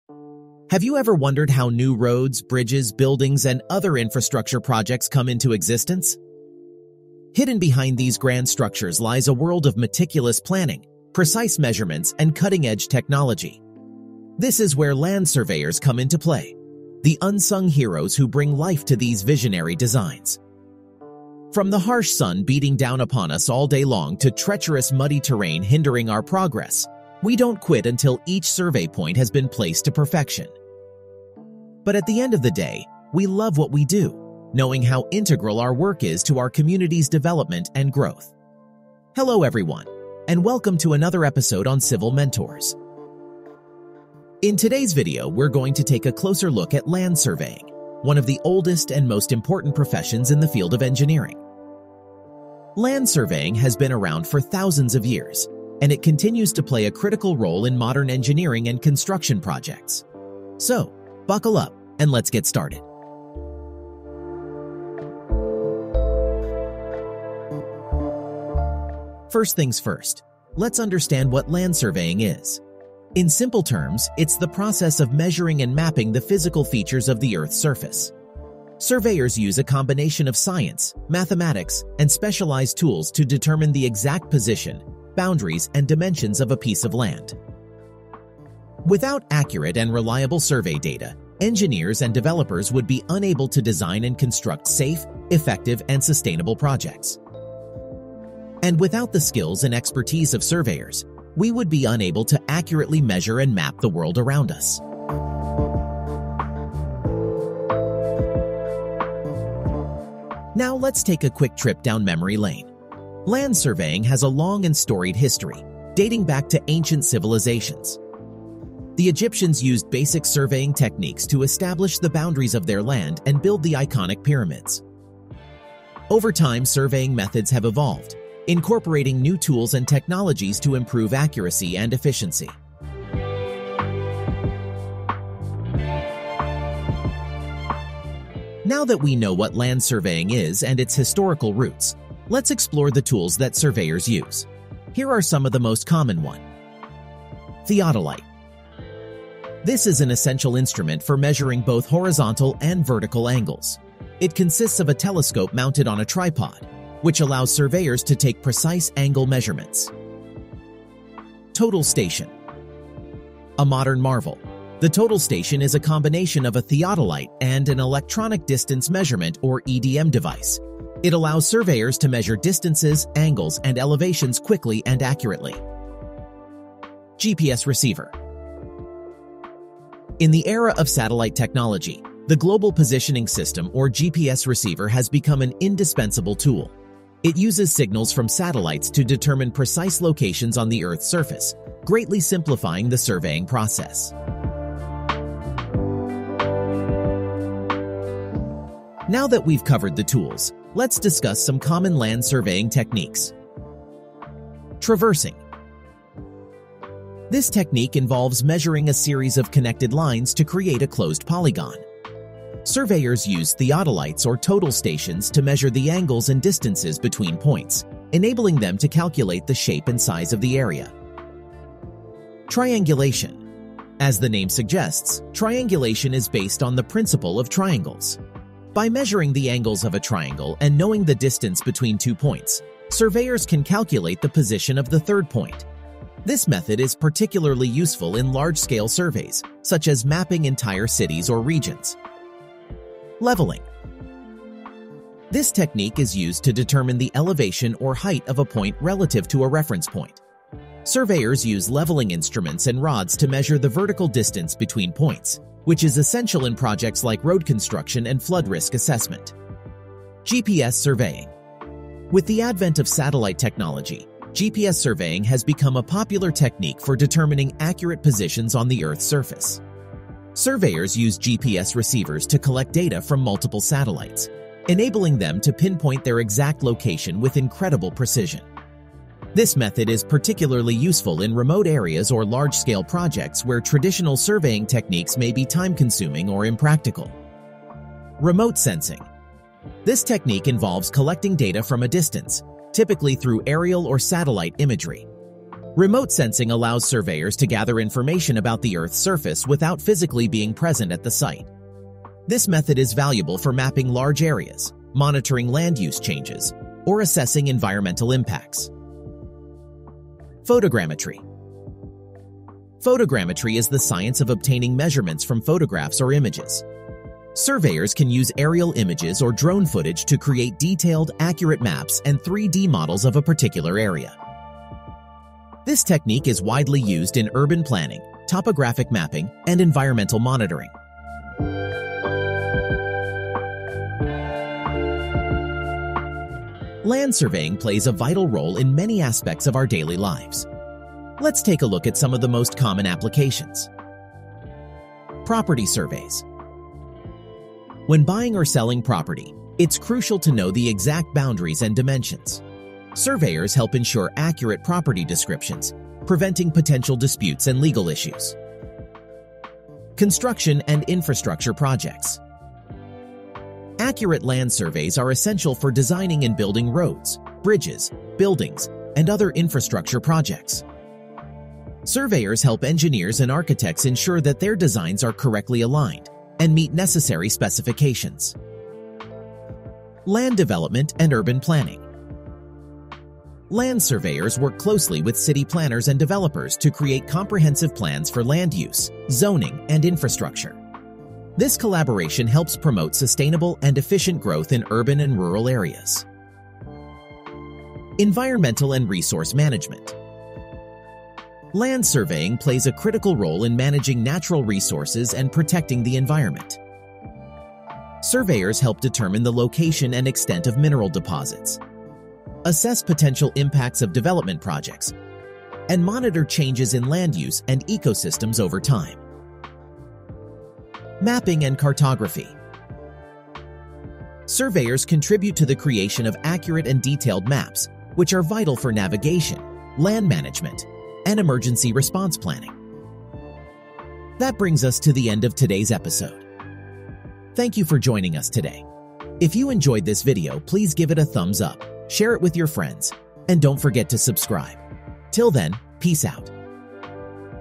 What is Land Surveying?
In today's video, we're going to take a closer look at land surveying, one of the oldest and most important professions in the field of engineering.

☑️☑️☑️Follow US ☑️☑️☑️